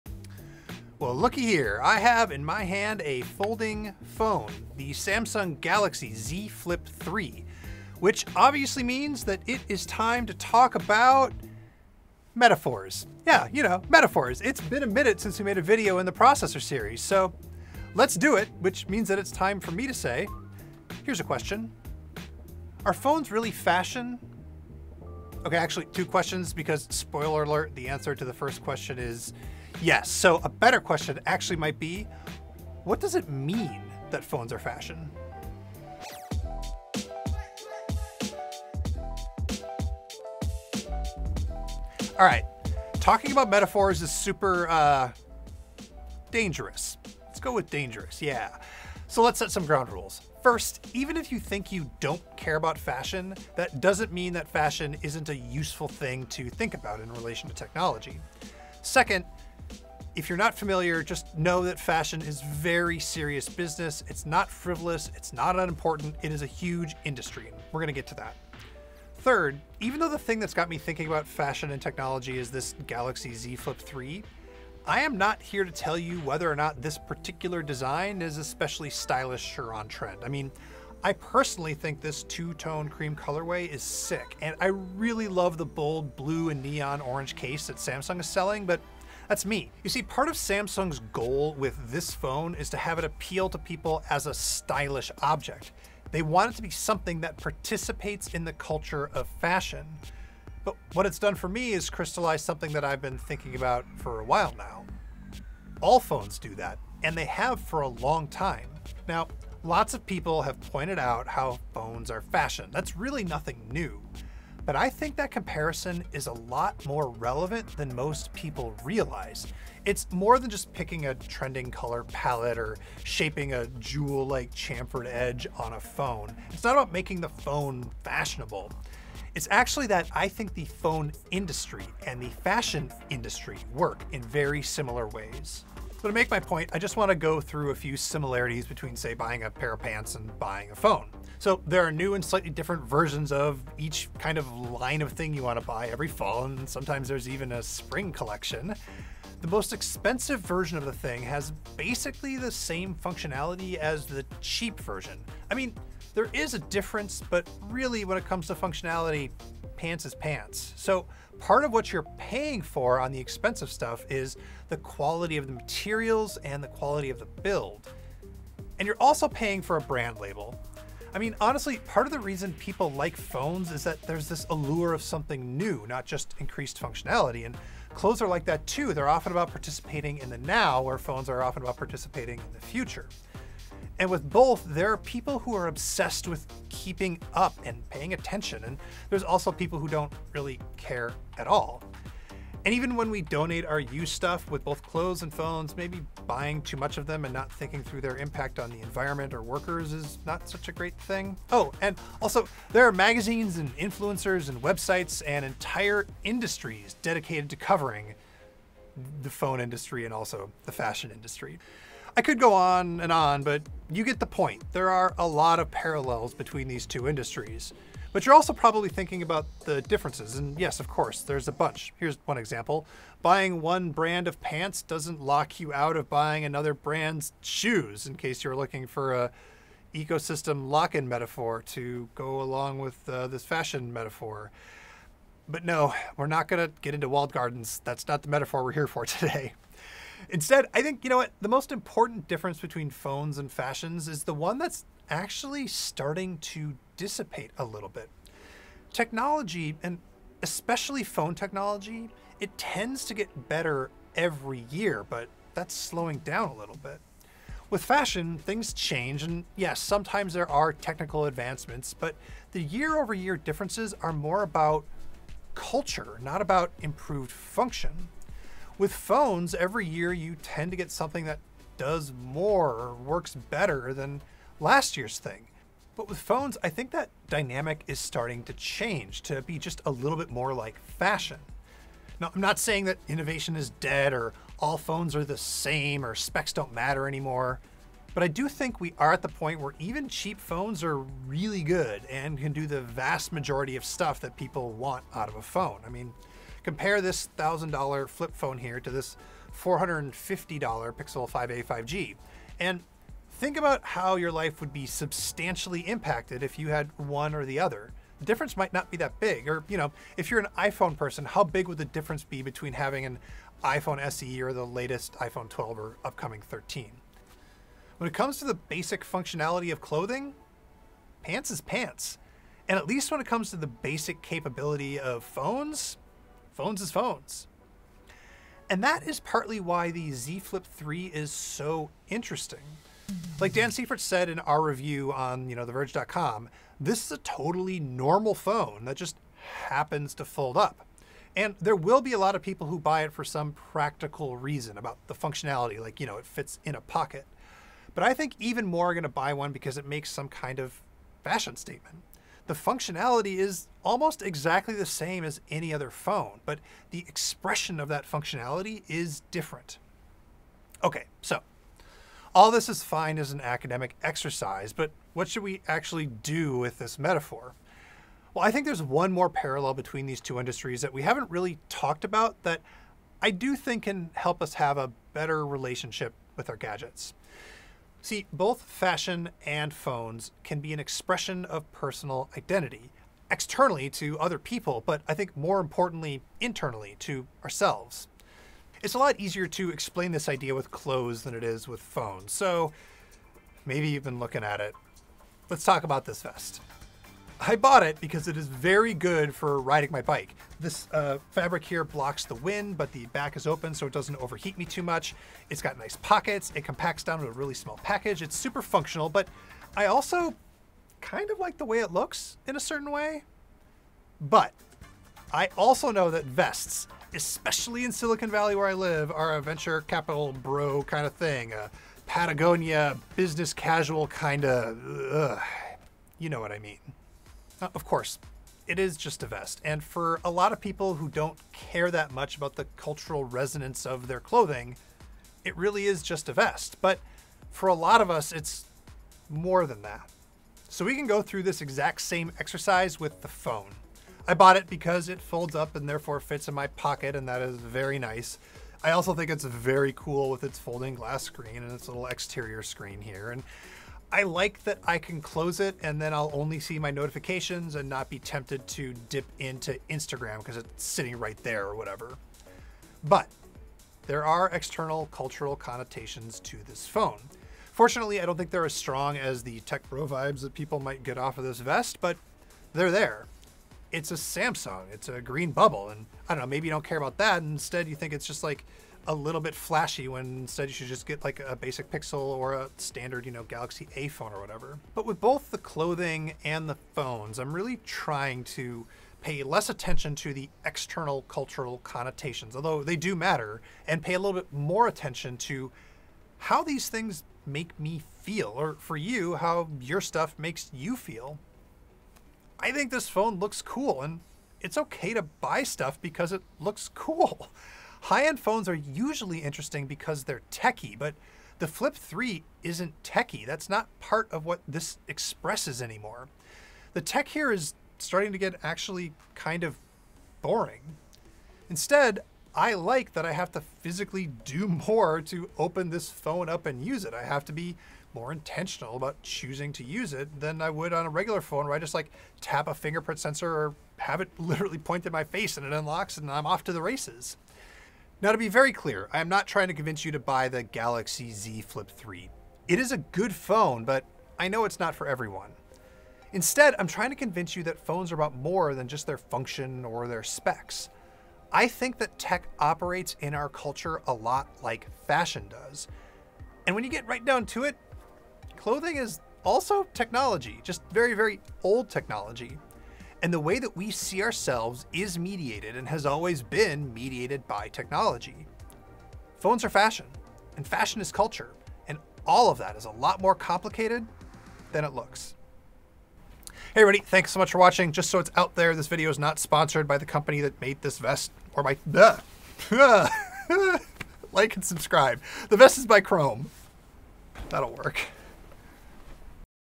Z Flip 3: actually it's fashion
The Samsung Galaxy Z Flip 3 is a totally normal phone with one weird folding trick. Now that it's crossed that barrier, why does it exist? Samsung wants the answer to be fashion, but in this return to the Processor video series, Dieter Bohn looks closer at what it means when phones try to be fashion.